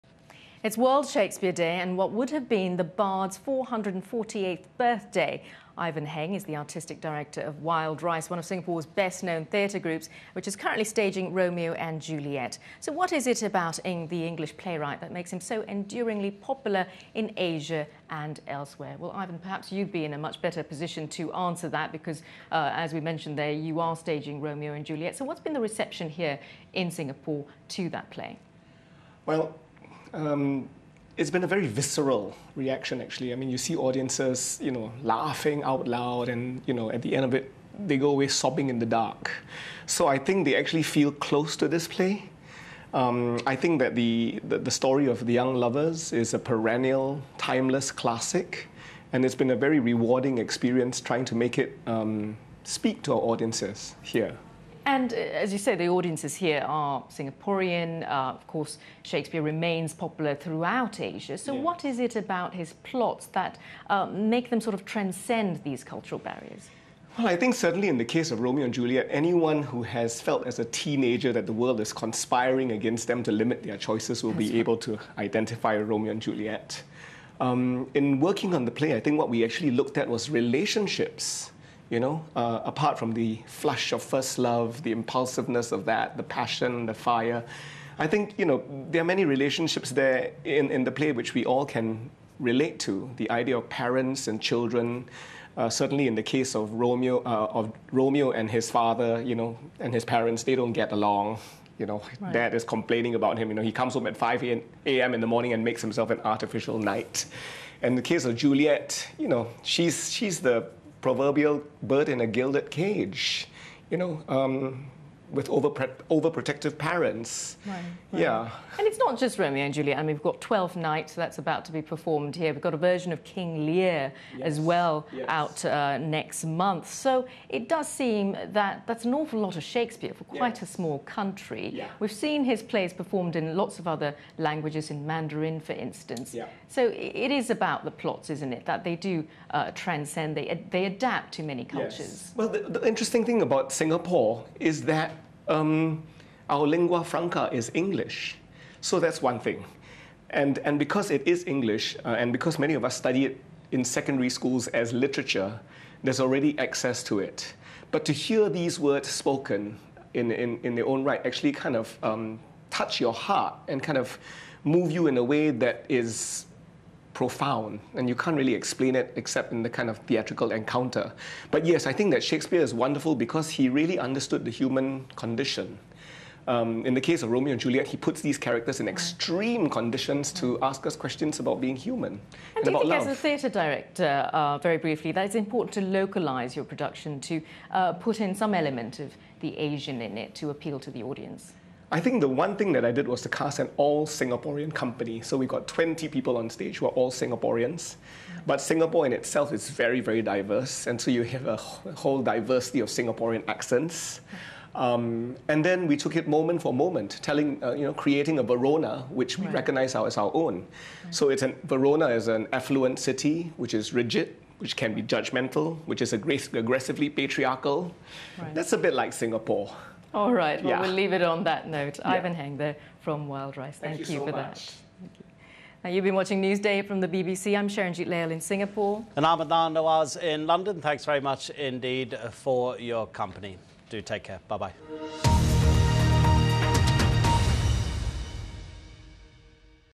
Ivan Heng on the BBC | World Shakespeare Day 2012
Sharanjit Leyl interviews Ivan Heng on World Shakespeare Day.
Broadcast on the BBC on 23rd April 2012.